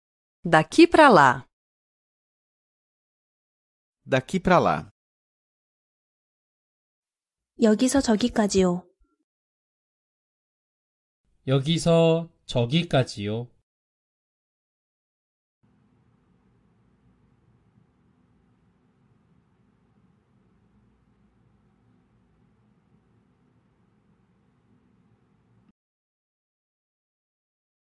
포르투갈어로 "여기서 저기까지요." 말하는 법
남자 목소리 :
Daqui pra lá.

여성 목소리 :
Daqui pra lá.

남성 음성 번역 :
여기서 저기까지요.